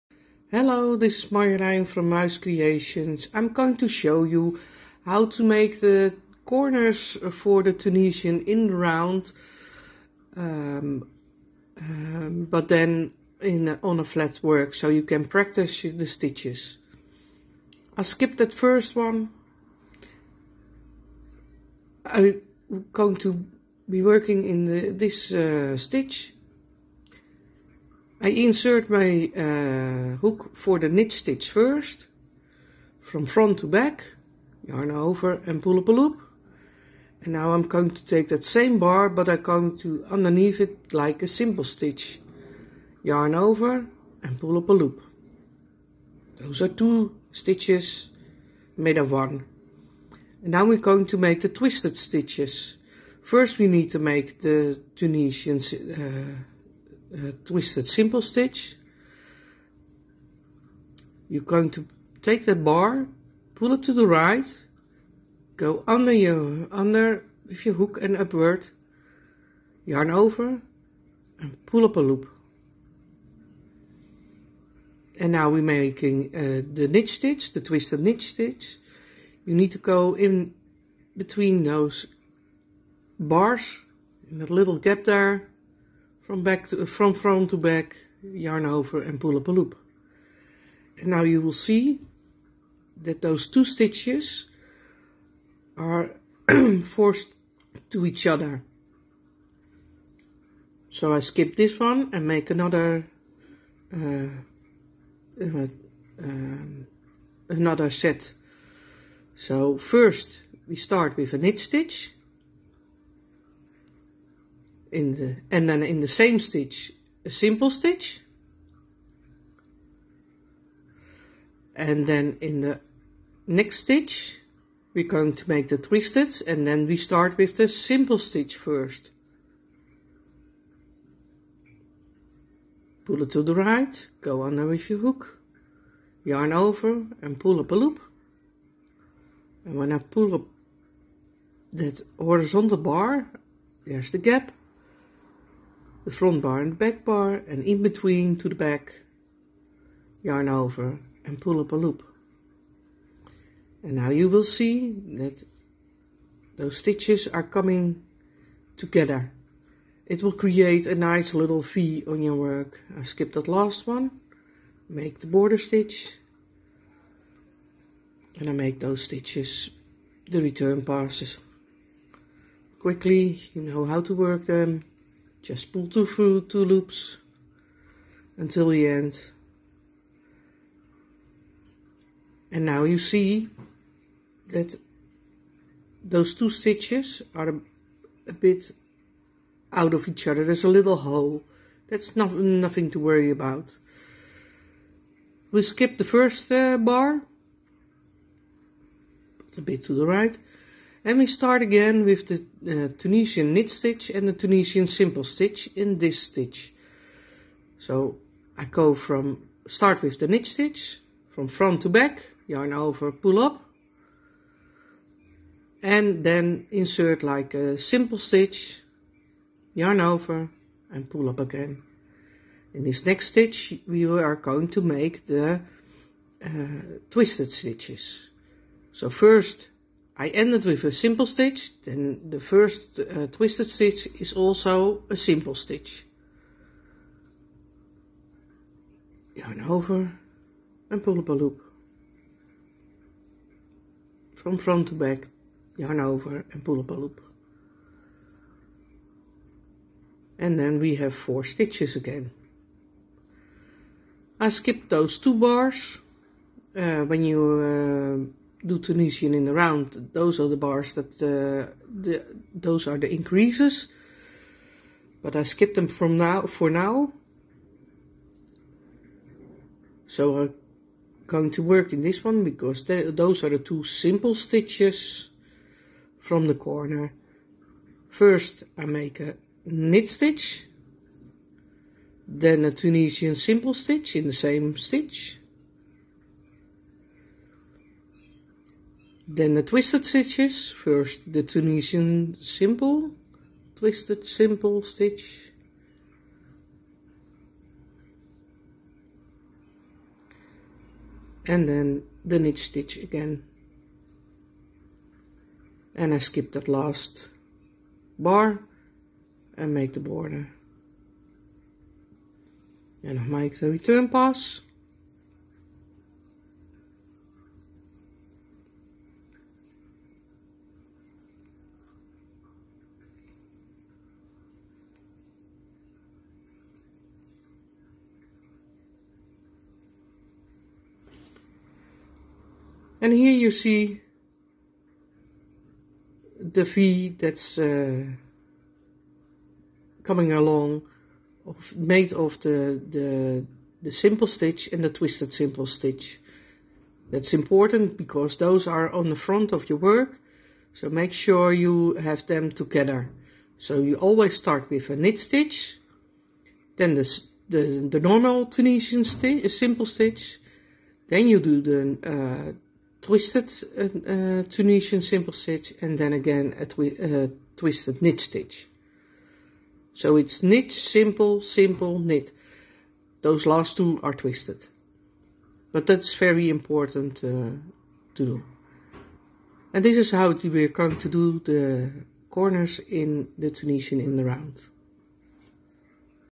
combining stiches corner
TKS/TSS in the same stitch and Twisted TSS/TKS in the same stitch next to each other.